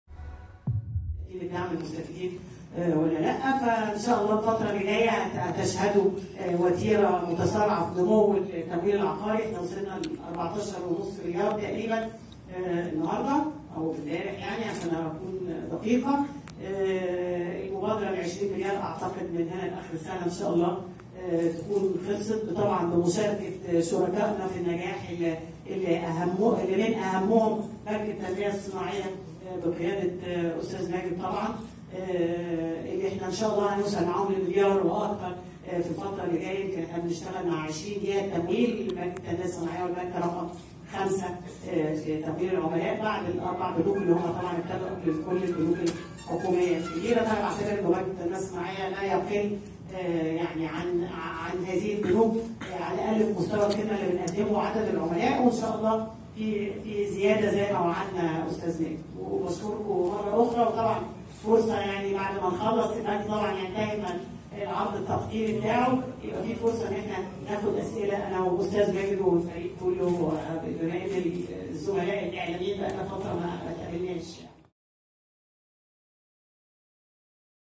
رئيس التمويل العقاري: 14.5 مليار جنيه للإسكان الاجتماعي
سيد محمد - تصوير مؤمن سمير
قالت مي عبد الحميد، رئيس صندوق التمويل العقاري، إن 14.5 مليار جنيه هو حجم التمويلات التي وفرتها البنوك لمستفيدى مشروع الدولة للإسكان الاجتماعي.
وأوضحت "عبد الحميد"، أن الصندوق أنهى إجراءات 156.5 ألف عميل حتى الآن، مشيرة إلى أن الصندوق يستهدف تسليم كافة حاجزى وحدات الإعلانات من 1 إلى 8 للمشروع بنهاية العام الجاري 2018.
وكان صندوق التمويل العقاري بوزارة الإسكان، وقع اليوم الثلاثاء، بروتوكول تعاون مع بنك التنمية الصناعية، ضمن مبادرة البنك المركزي لدعم التمويل العقاري للمواطنين محدودي الدخل.
ويستهدف البروتوكول تمويل شريحة جديدة من المستهدفين بمشروع الإسكان  الاجتماعي لمحدودي الدخل.